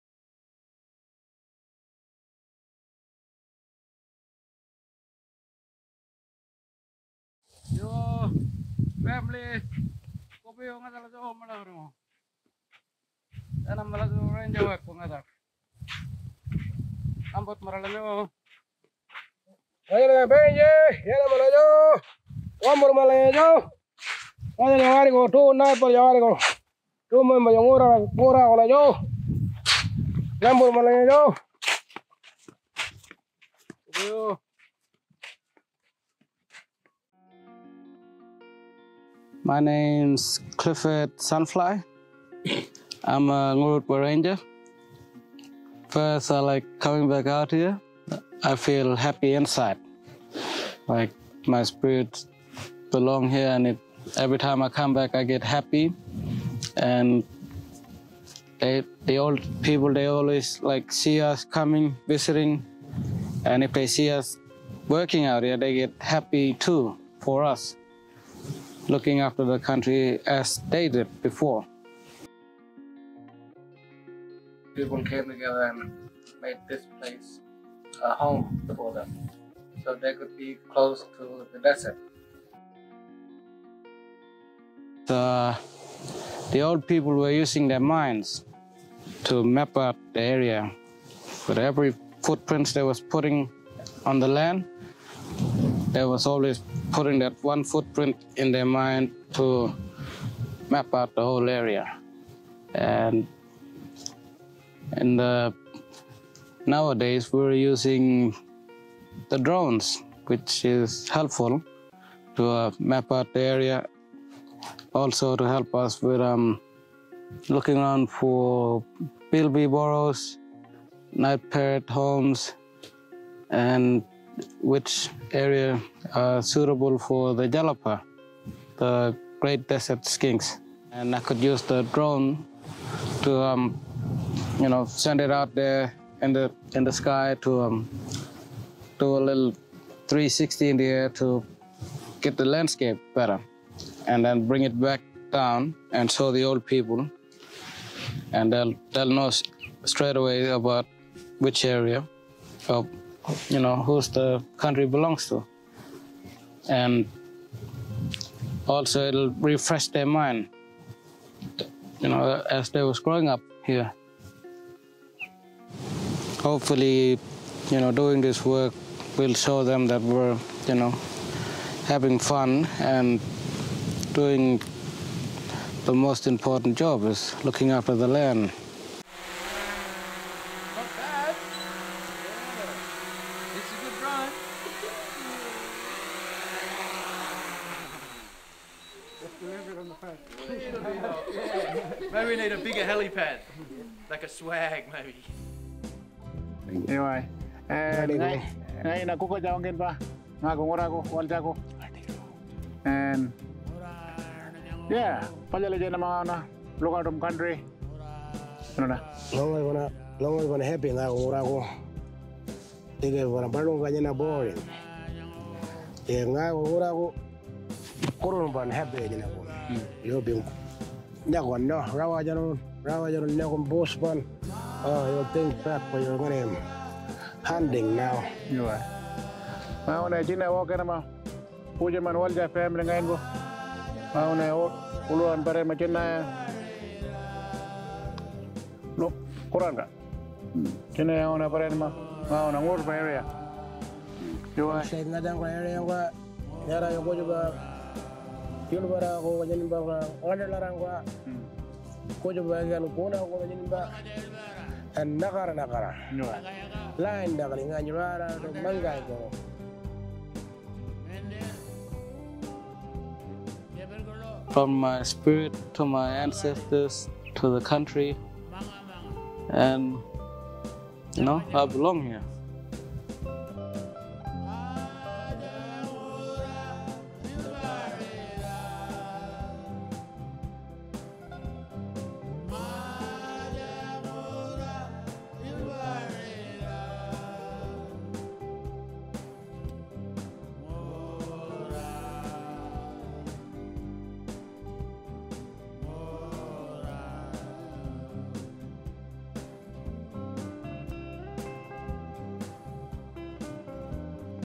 Ngurra Yulpararra
A film by the Ngururrpa Rangers about looking after country using drone mapping.
“Our elders were using their minds to map out country, nowdays we have the drone to map out country” - Clifford Sunfly, Ngururrpa Rangers
The Ngururrpa Rangers trained with Staf Smith from Right Place Geospatial at Balgo Community and Yagga Yagga in Western Australia to develop their drone operation skills. This was a big step forward for the Ngururrpa Rangers who are now planning to use a drone to map out landscapes and support their work looking after threatened species.
This film was created by Kerenza Sunfly, a young Ngururrpa man, in collaboration with the Ngururrpa Rangers and the Indigenous Desert Alliance. Kerenza was inspired to show how rangers are using drones to map country the right way. Kerenza says that “operating drones out on country has been really important and has become very popular these past few years because it lets rangers see country like they’ve never seen before, it’s very useful and also very helpful to our elders.”

Kerenza works with the media and ranger development teams at the Indigenous Desert Alliance.